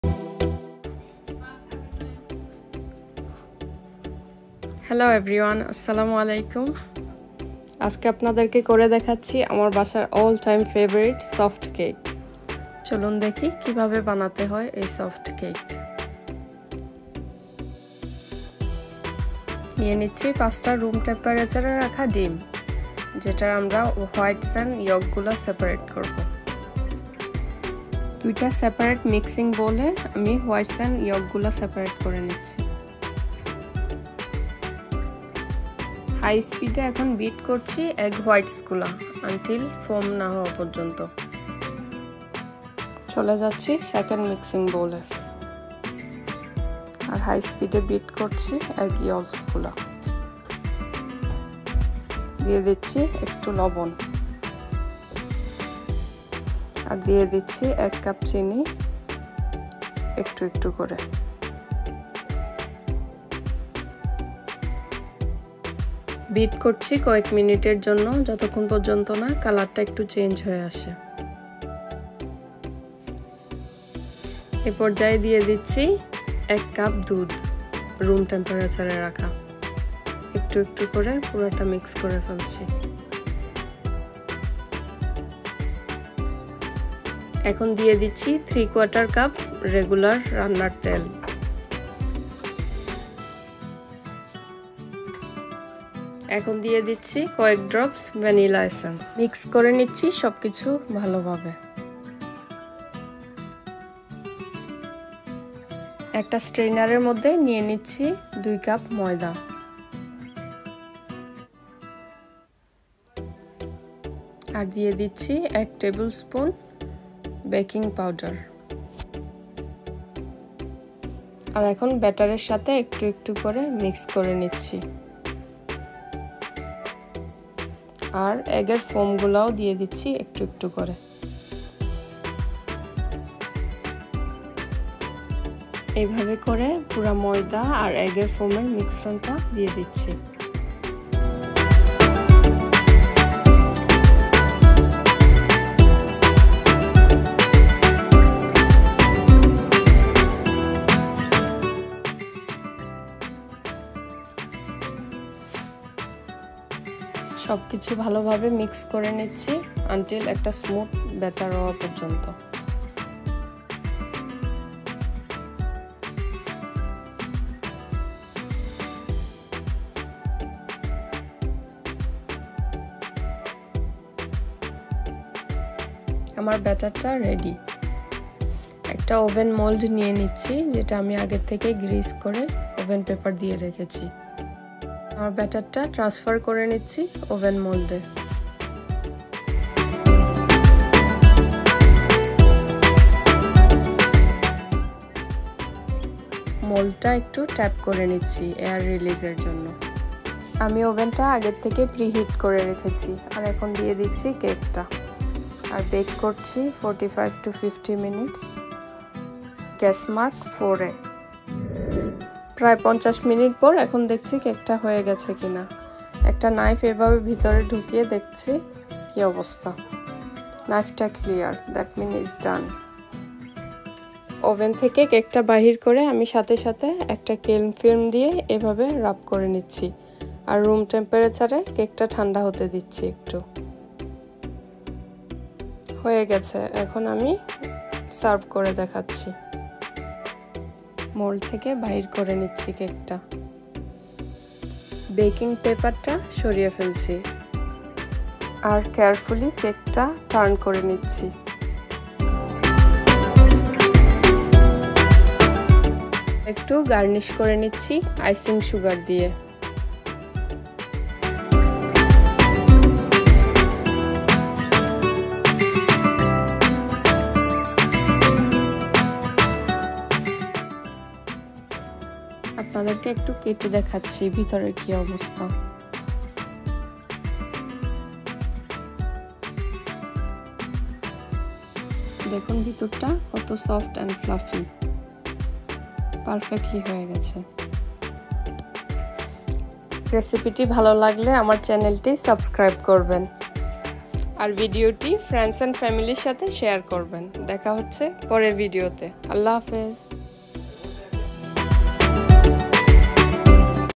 How to make Soft cake | নরম কেক
Ingredients for the soft cake
- 5 medium eggs
- 1/2 tsp of salt
- 1 cup of sugar
- 1 cup of milk
- 3/4 cup of vegetable oil
- 2 cups of plain flour
- 1 tbsp of baking powder

music credit

Beatbyshahed